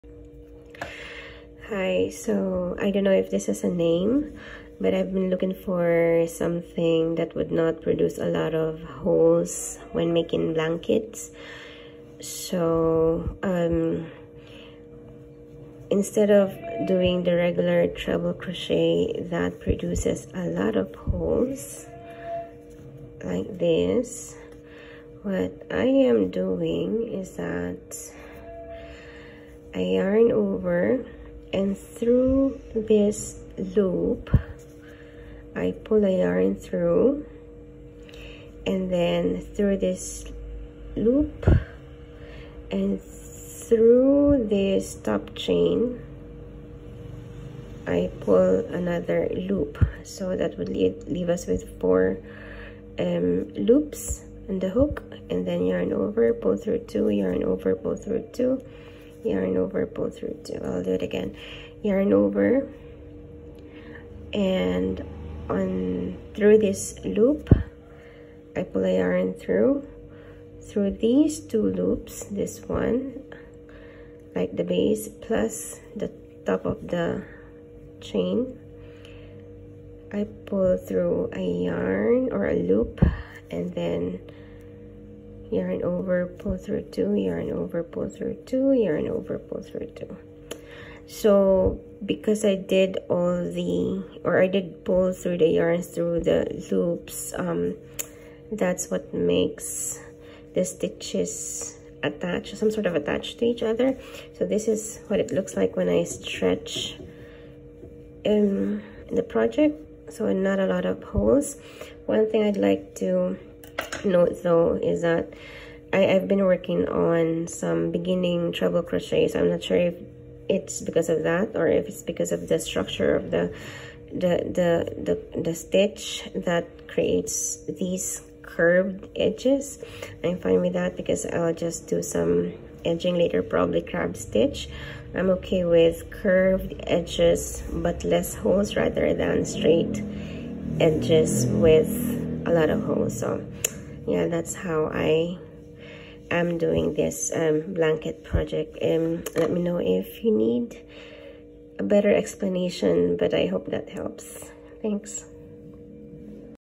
Modified Treble (US) Crochet
Uploading this video for those who asked through one of the Crochet groups via Facebook.

I made this stitch because I wanted to have less holes on my blanket project.

For this project, I am using double pull of acrylic fingering yarn called “lanalon”.